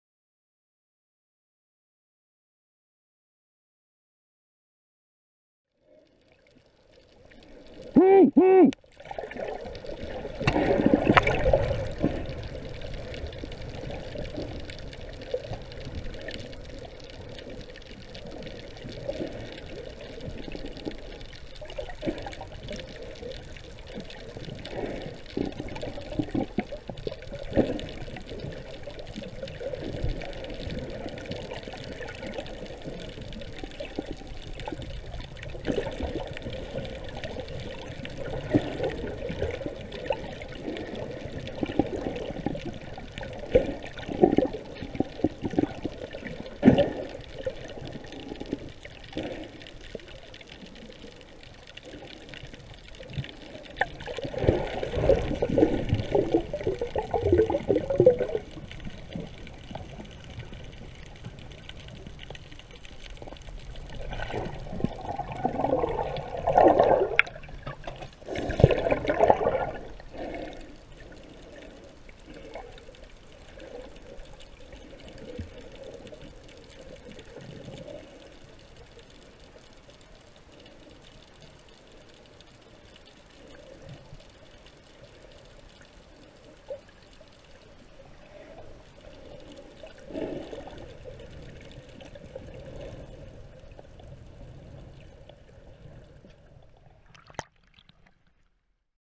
Snorkeling with a green sea turtle in Navarre Beach, FL Brad Coke Emily Coke
Snorkeling with a green sea turtle in Navarre Beach, FL. Brad and Emily Coke